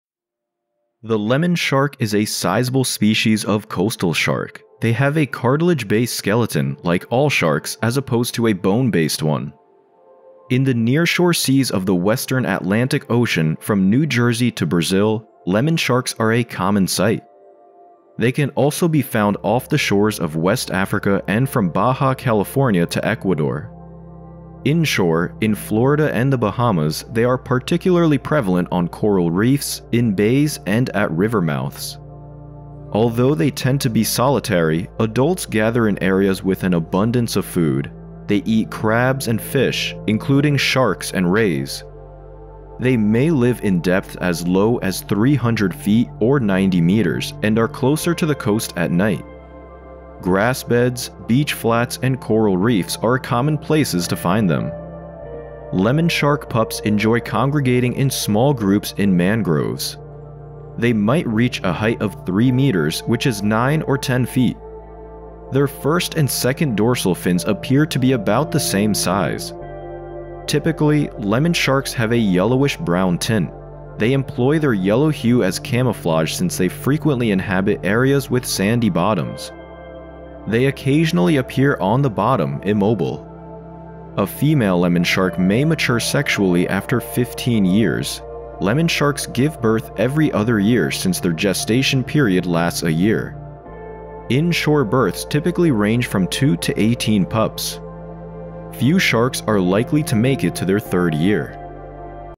ALL ABOUT THE LEMONSHARK
The lemon shark is a sizable species of coastal shark.

Learn all about this shark in an educational video.